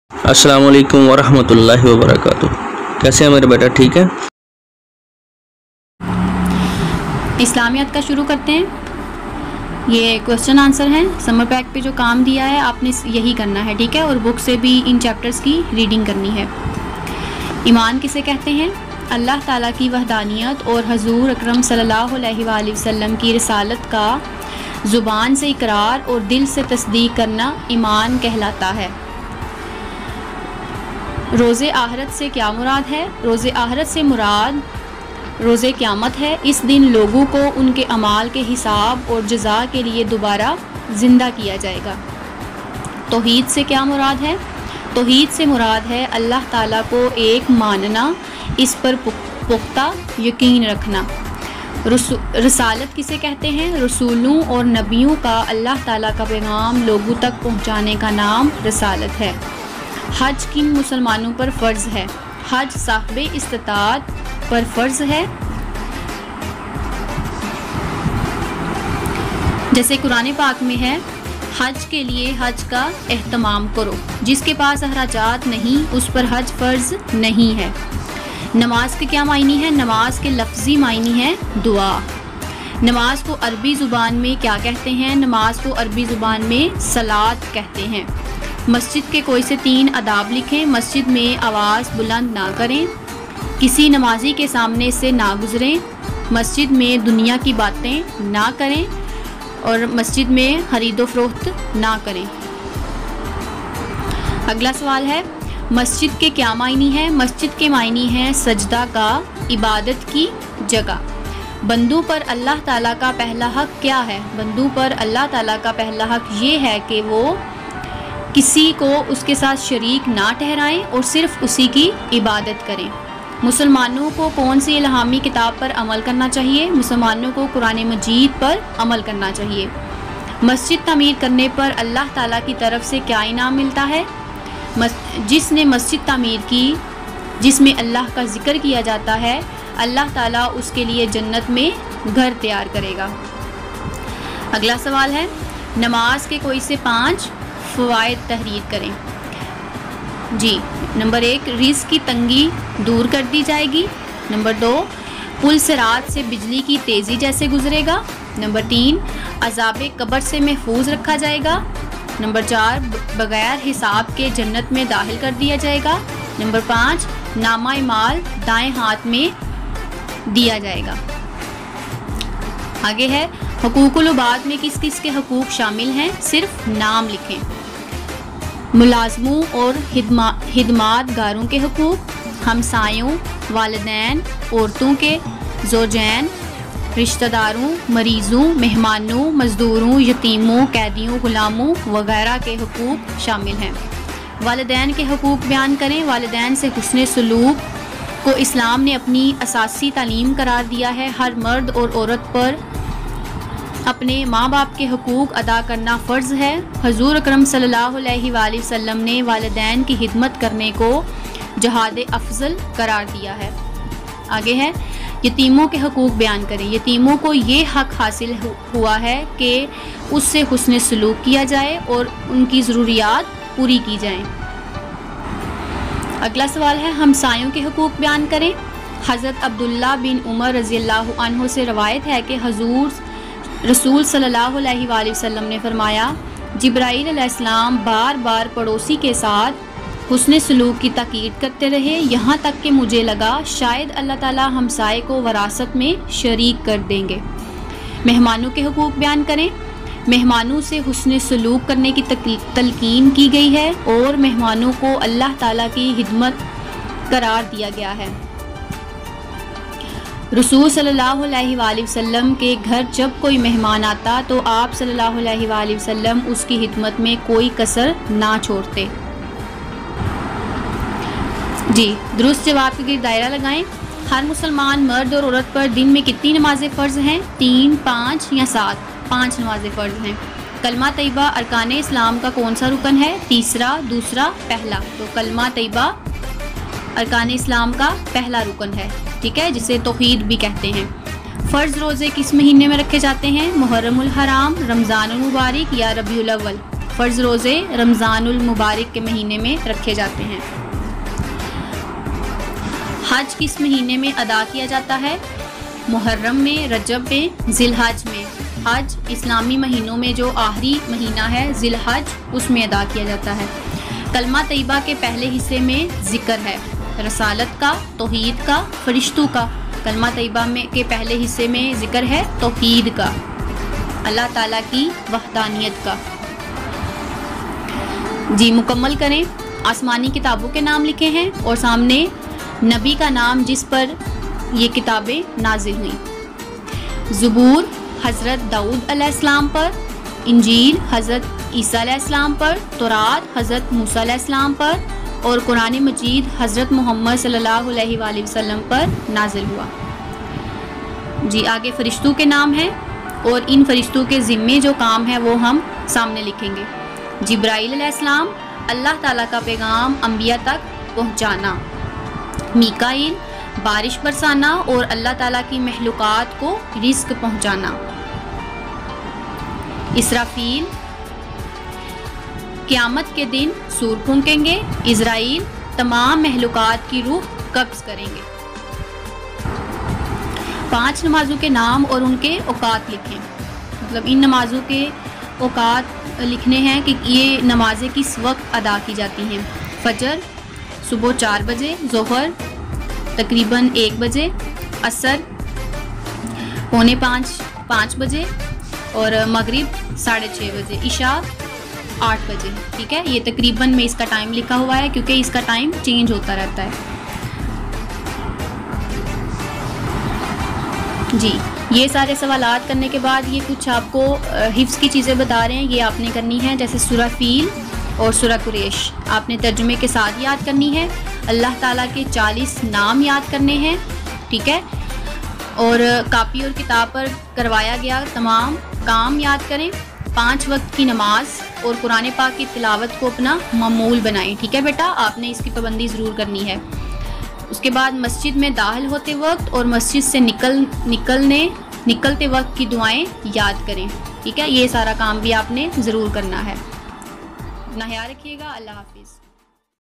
Class 4 islamiyat complete summer pack April 17, 2020
in this video you'll learn about Toheed Rasalat,  Frishtay,  ilhami kitabein, roz w akhrat, hajj, masjid, nimaz and haqooq ul ebad